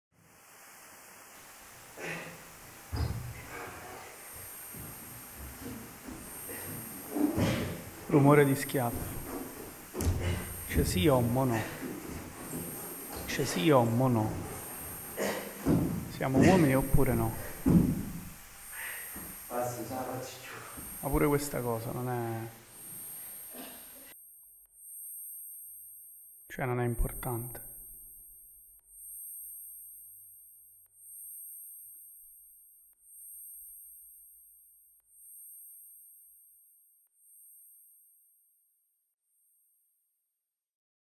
TVATT / Trasparenze trailer
TVATT
TEORIE VIOLENTE APRIORISTICHE TEMPORALI E TERRITORIALI
Uno spettacolo ideato da Lugi Morra liberamente ispirato a “East” e “West” di Steven Berkoff

Con: Luigi Morra, Pasquale Passaretti, Eduardo Ricciardelli
Musiche e suono: Camera
Luci e video: Domenico Catano
Elementi scenici: Stefano Zecchini
Drammaturgia e regia : Luigi Morra